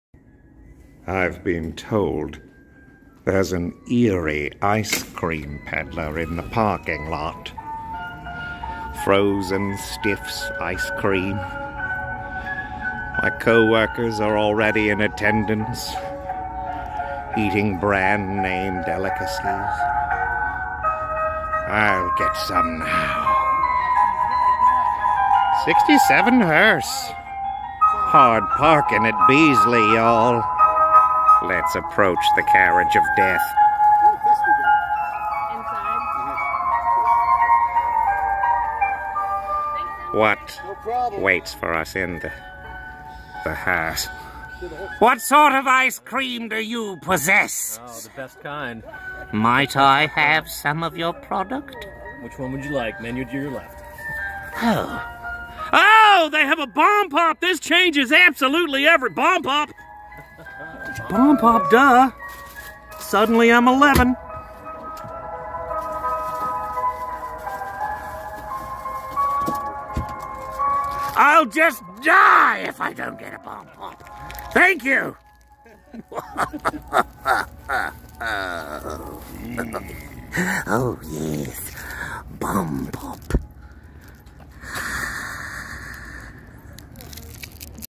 LAST RIDE, WITH SPRINKLES
Frozen Stiffs Ice Cream stopped by today. I got a Bomb Pop, which was awesome! Their Hearse is a gorgeous 1967 Cadillac with a 429. Such a beautiful car!